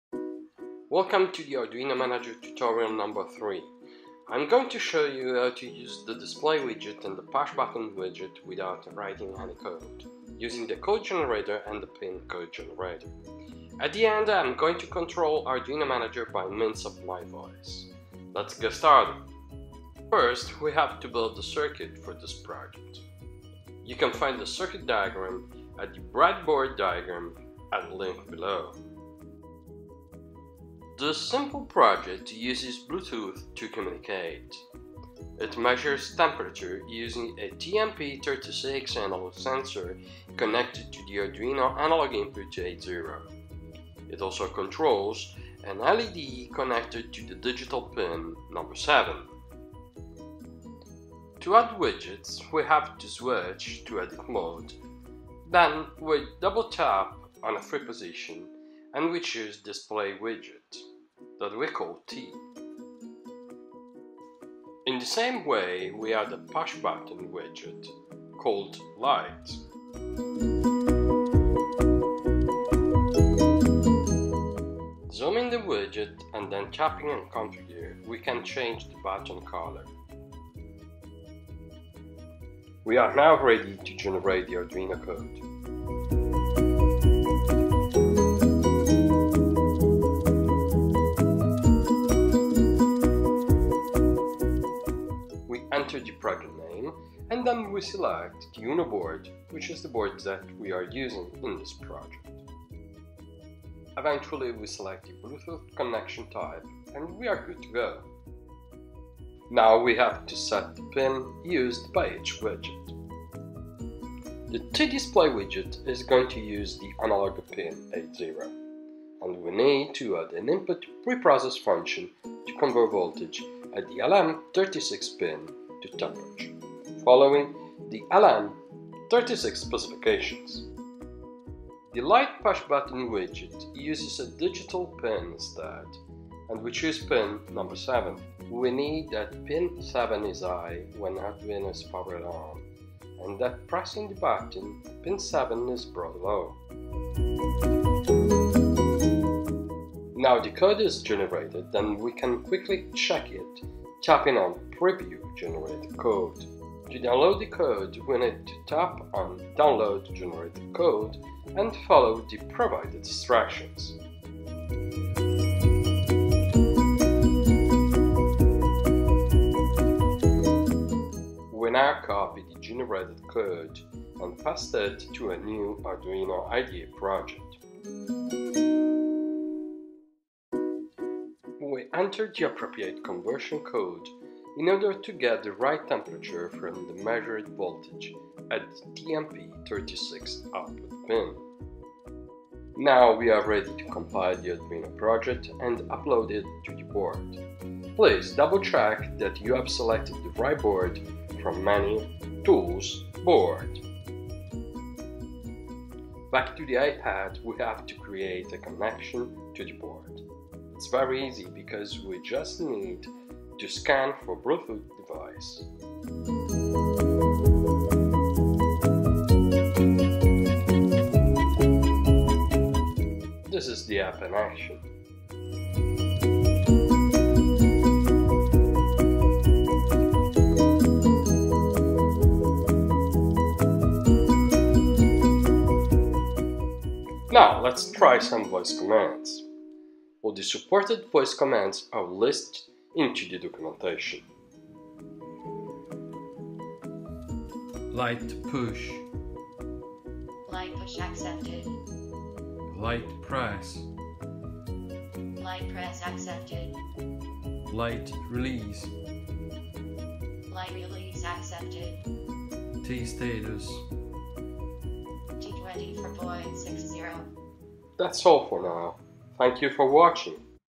Tutorial 03
Welcome to Arduino Manager tutorial #3. In this tutorial I'll show you how to use the Bluetooth connection, Display Widget and Push Button Widget without writing any code and voice commands.